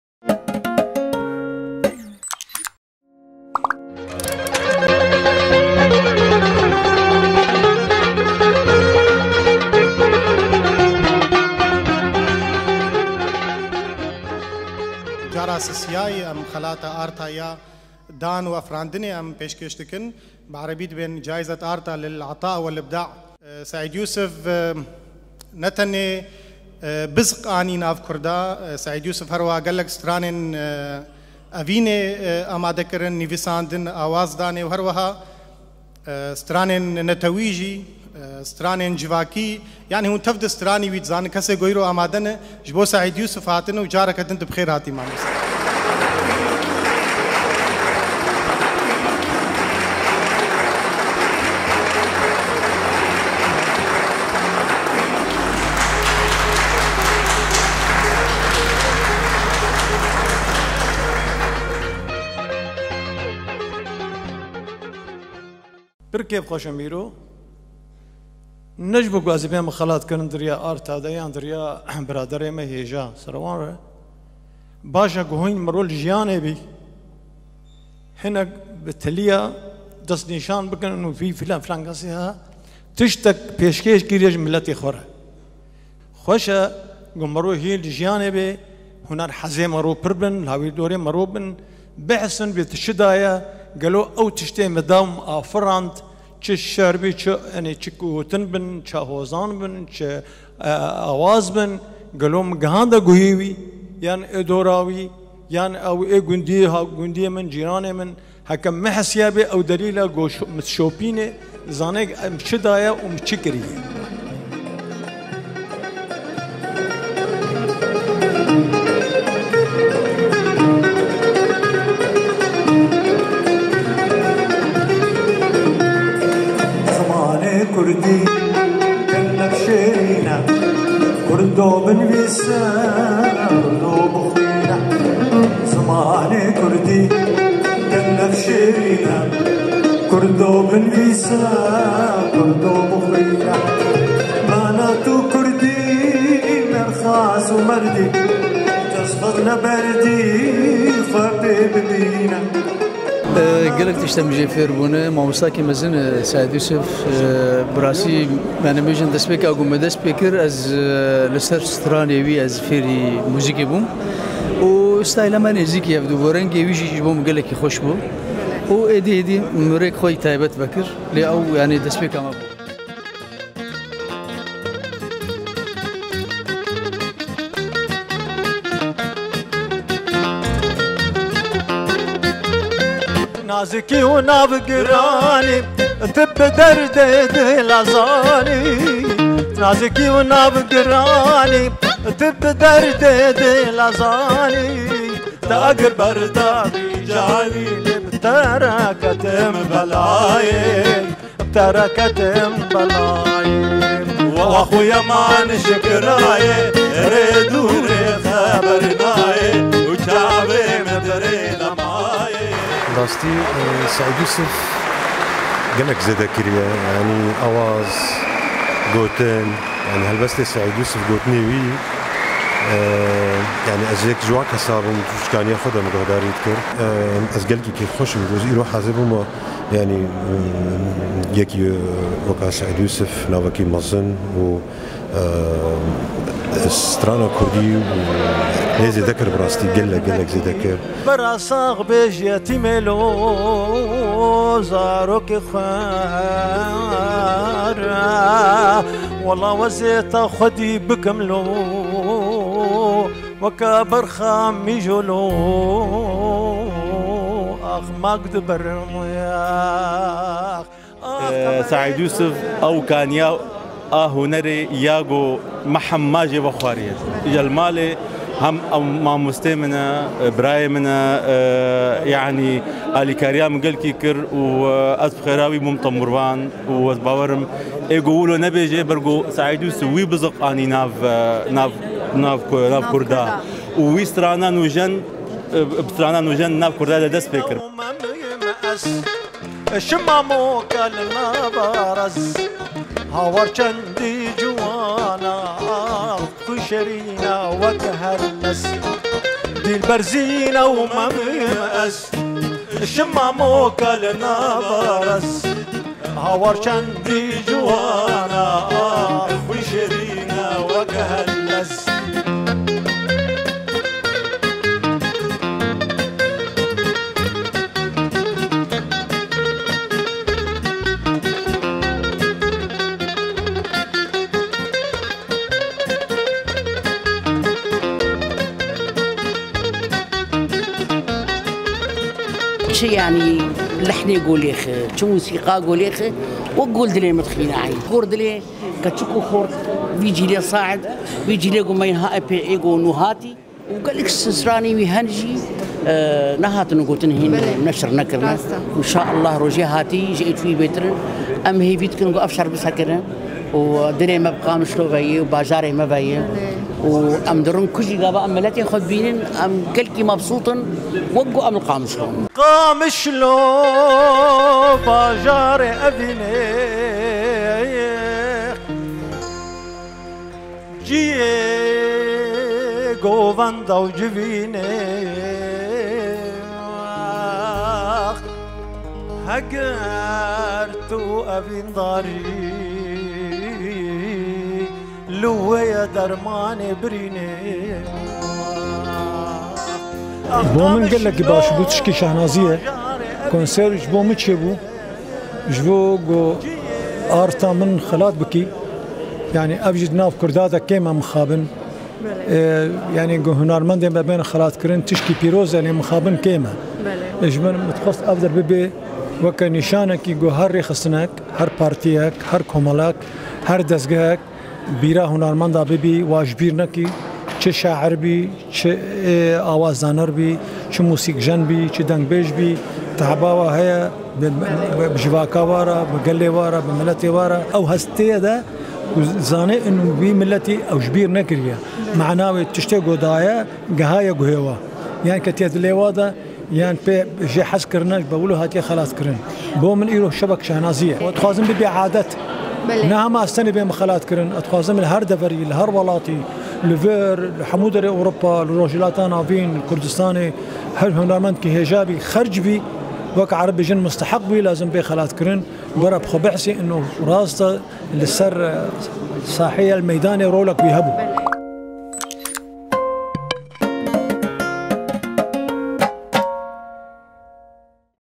حفل تكريم الفنان سعيد يوسف في ألمانيا
مقتطفات من حفل تكريم الفنان القدير سعيد يوسف بجائزة آرتا للعطاء والإبداع في مدينة بوخوم بألمانيا. استمتعوا بالأجواء الموسيقية الجميلة.